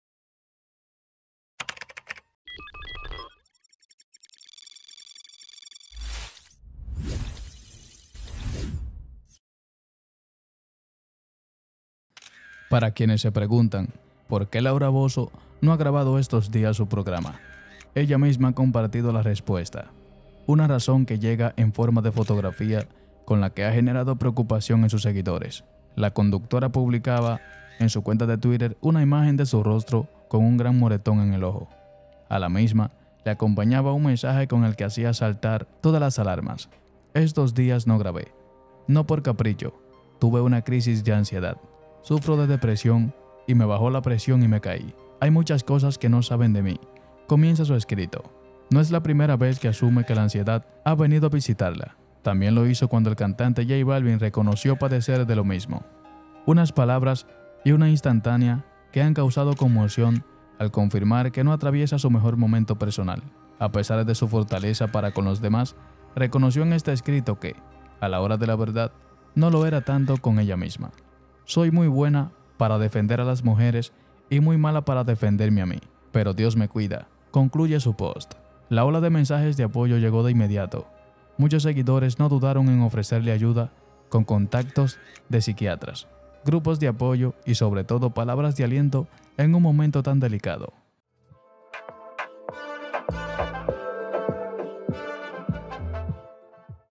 Laura Bozzo SUFRE CAÍDA en medio de un ataque de ANSIEDAD y se GOLPEA la cara
La presentadora publicó una foto con el rostro amoratado y un mensaje que hizo saltar todas las alarmas: "Sufro depresión... Hay muchas cosas que no saben de mí".